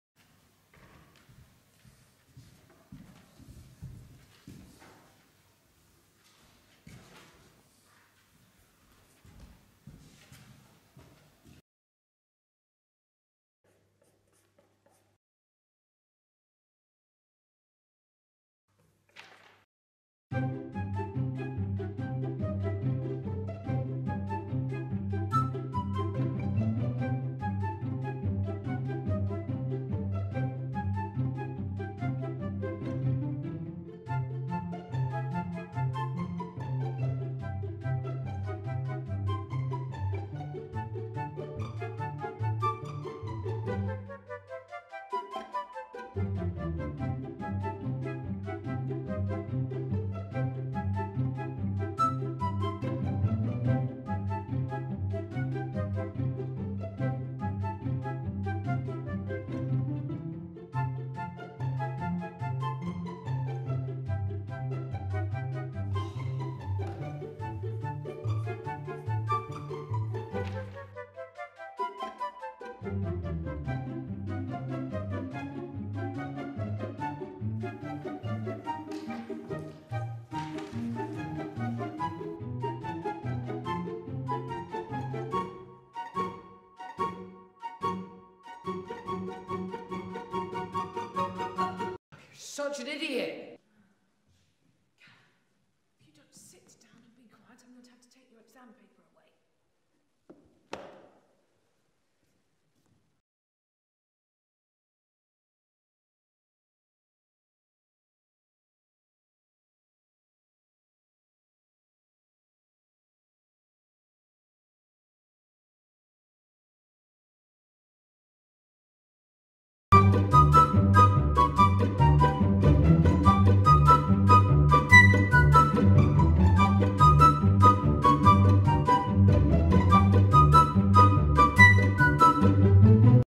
Talking to Yourself - Short Film
I put together a short film with a bunch of people from school recently.
I was going to try and do a lot of experimenting with cloning but that didn't really work because of the sun going back and forth behind the clouds... I'm fairly pleased with the outcome though!